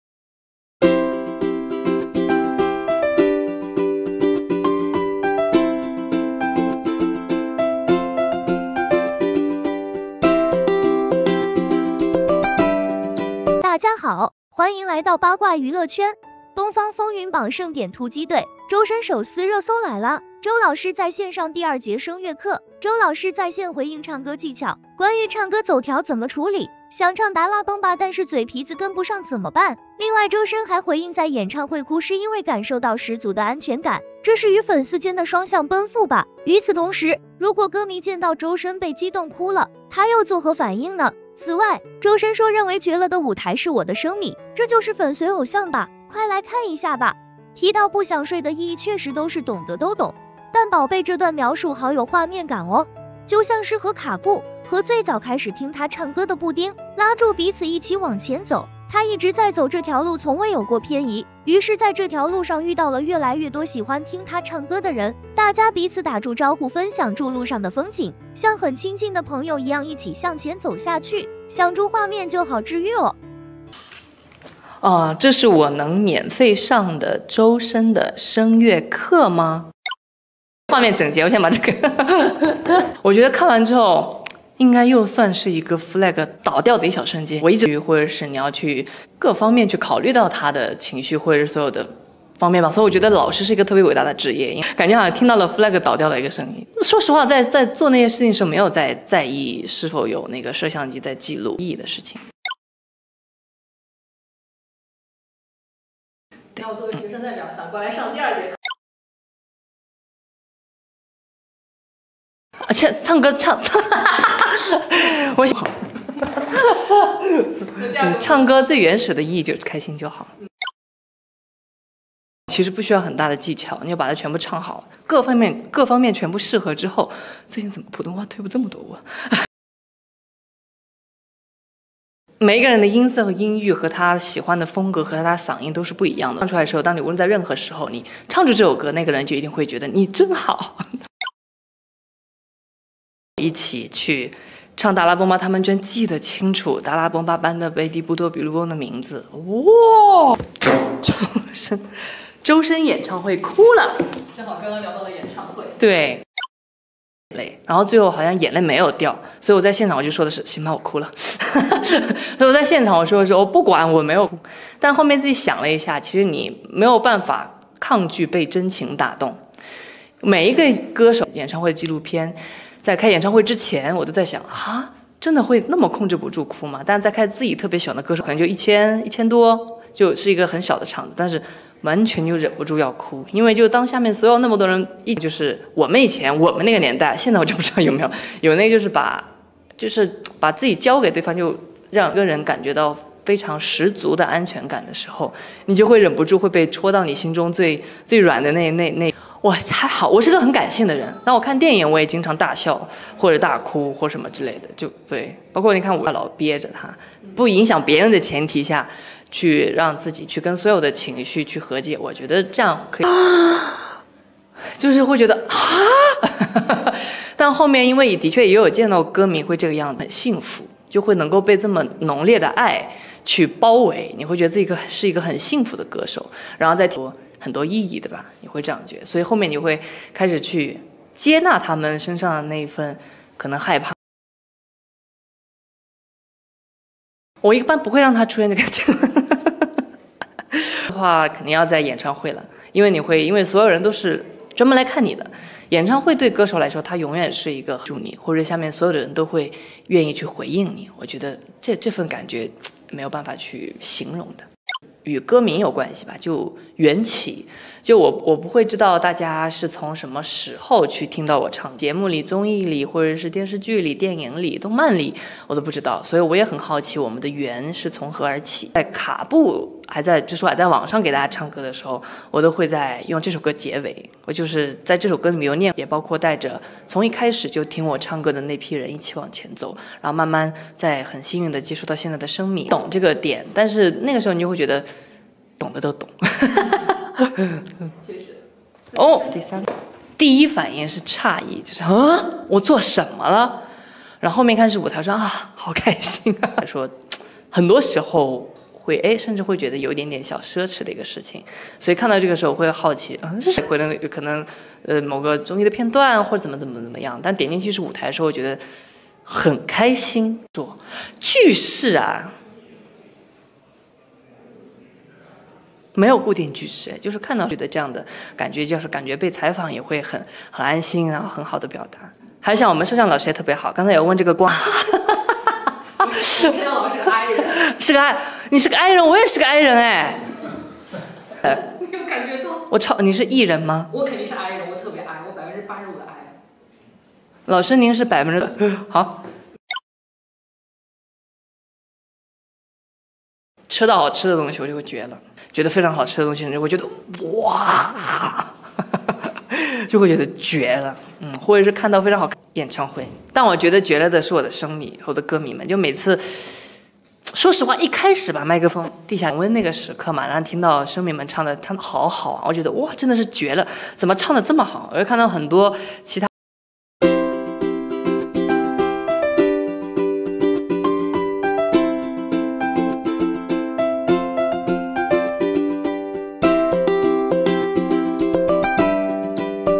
周深手撕熱搜來啦，周老師在線上第二節聲樂課，這段描述好有畫面感！
周深手撕熱搜來啦，周老師在線上第二節聲樂課，這段描述好有畫面感！周深 Charlie Zhou Shen